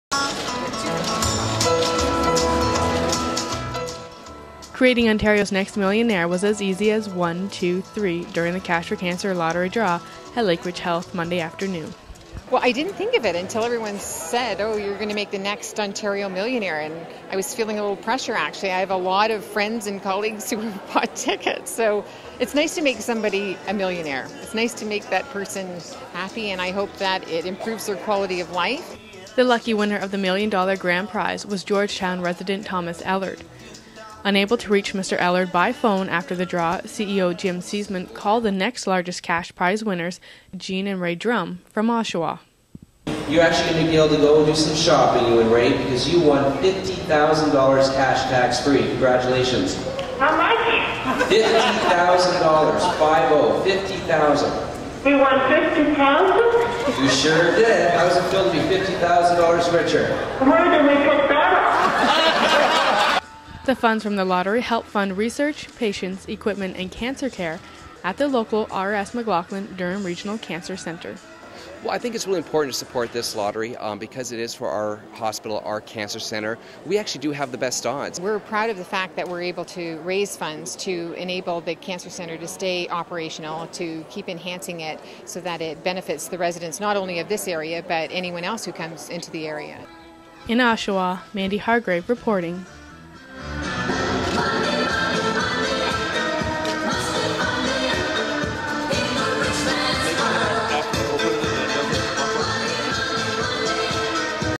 A Phone Call She Will Not Forget
Jean Drumm could not believe her ears when Jim Szeman called her on Monday. The CEO of the Lakeridge Health board called Mrs. Drumm to tell her she won $50,000 in the Cash for Cancer lottery, and it took a little convincing as Mandi Hargrave reports.